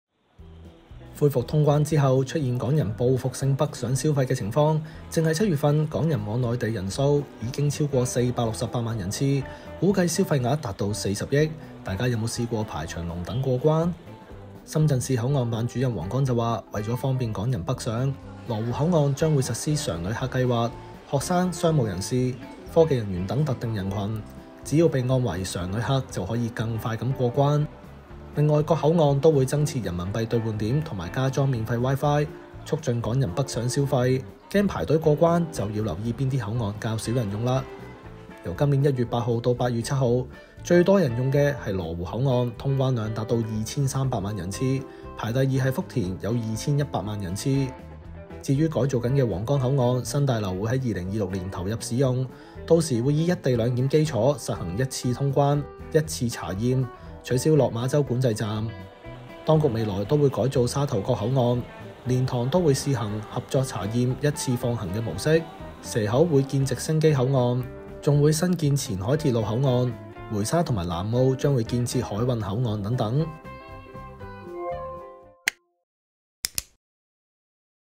深圳口岸辦：羅湖將實施「常旅客」計劃 採措施便利港人北上消費｜01新聞｜羅湖｜深圳｜通關｜過關
深圳口岸辦：羅湖將實施「常旅客」計劃　採措施便利港人北上消費

深圳市口岸辦主任王剛表示，今年初恢復通關後，羅湖口岸仍是各口岸旅檢通關量第一名。由1月8日至昨日（8月7日），羅湖通關量為2300萬人次。他指，當局擬提升羅湖口岸的設施，並將實行「常旅客」計劃，學生、商務人士、其他科技人員等特定人群，只要備案為「常旅客」，通關查驗更加便捷；同時在各口岸設人民幣兌換點及加裝免費wifi，便利港人北上消費。